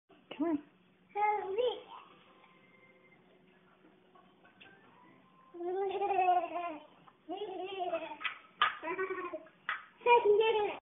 Gianni walks some more
April 23, 2013 - more walking around yiayia's den